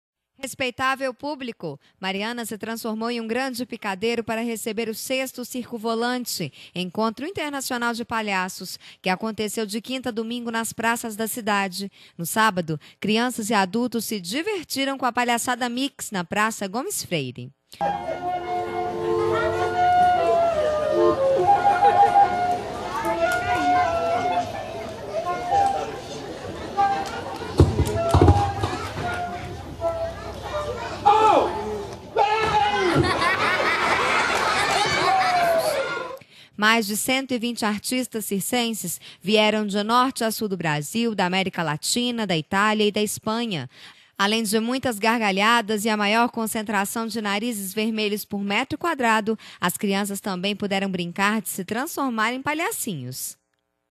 6º Encontro Internacional De Palhaços Do Circovolante
E o fim de semana em Mariana também foi animado com o sexto encontro internacional de palhaços do Circovolante!